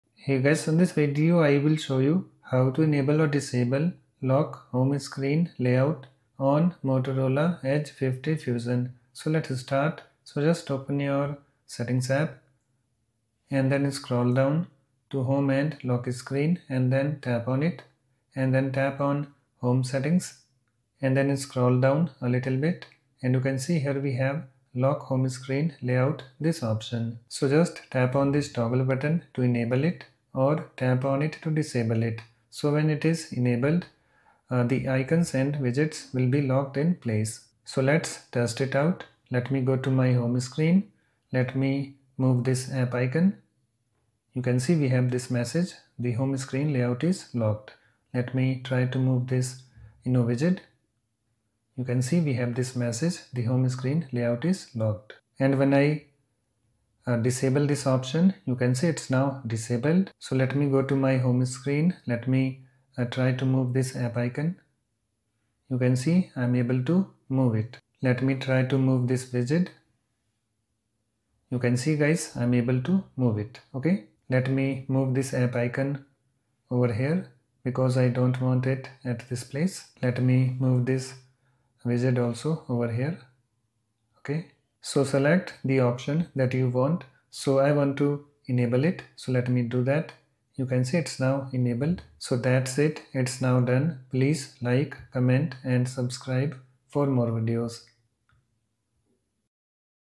How to Enable/Disable Lock Home Screen Layout on Motorola Edge 50 Fusion?
Hi guys, In this Video, I will show you "How to Enable/Disable Lock Home Screen Layout on Motorola Edge 50 Fusion."

How to Enable Developer Options on Motorola Edge 50 Fusion?